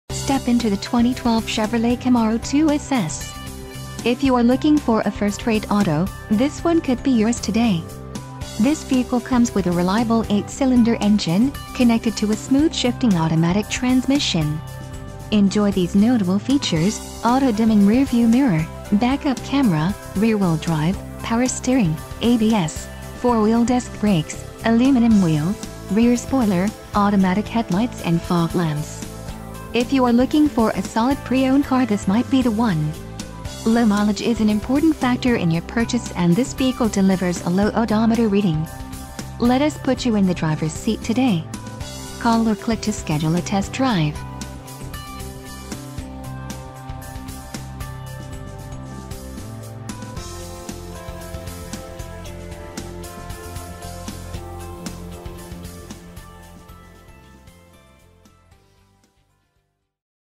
2012 Chevrolet Camaro 2SS - AutoMatch - Chicago - Morton ...
**CLEAN CARFAX**, **MOONROOF / SUNROOF**, **ONE OWNER**, and 2012 Chevrolet Camaro SS in Carbon Flash Metallic vehicle highlights include. 45th Anniversary Package, Brilliant Red/Medium Charcoal Stripe Package, and RS Package (Body-Color Roof Ditch Molding and High-Intensity Discharge Headlamps). Be the talk of the town when you roll down the street in this sizzling 2012 Chevrolet Camaro. There aren't any used vehicles more reliable than a Chevrolet, unless it's a Chevrolet with low mileage like this 2012 Camaro.